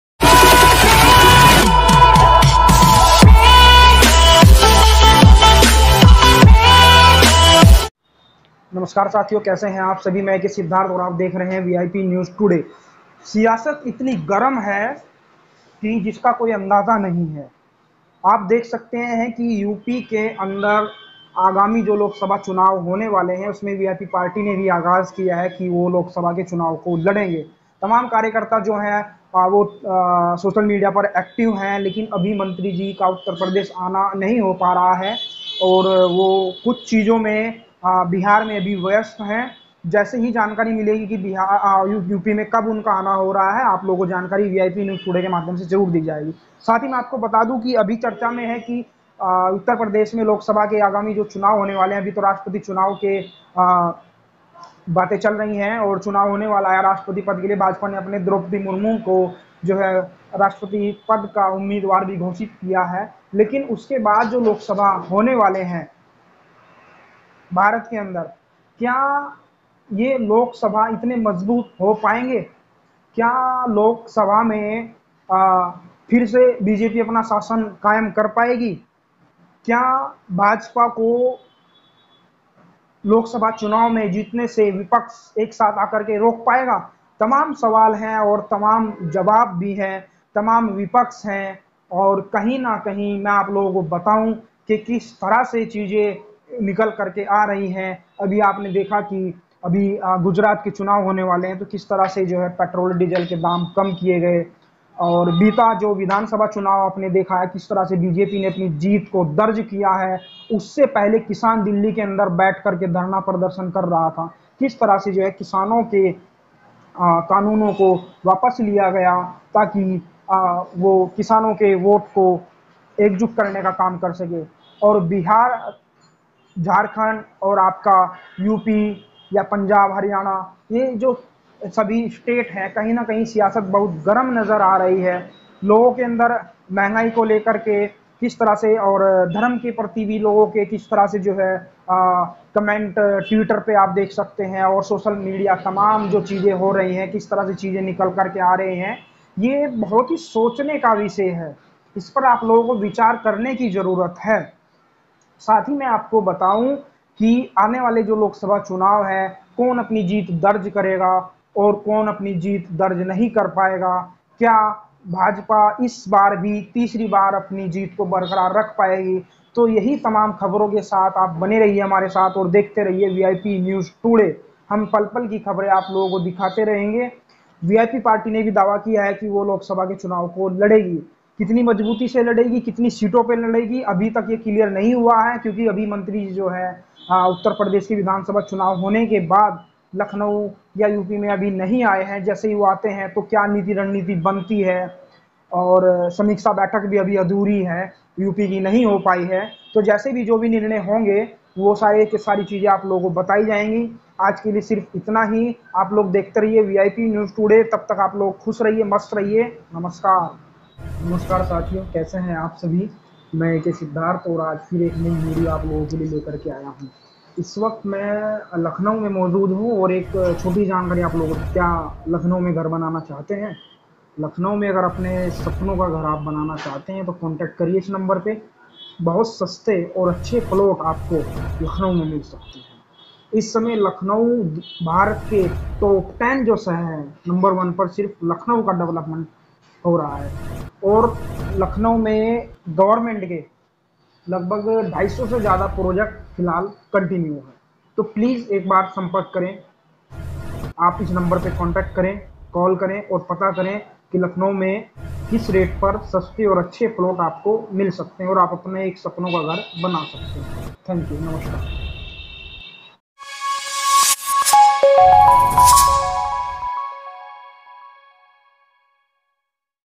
2024 में होने वाले लोकसभा में कौन किस पर भारी पड़ेगा क्या भाजपा फिर से सत्ता में आ पाएगी ? VIP News
2024 में होने वाले लोकसभा में कौन किस पर भारी पड़ेगा क्या भाजपा फिर से सत्ता में आ पाएगी ? VIP News

Phonepe
Google Pay
Paytm

Reporter- A.K.Siddharth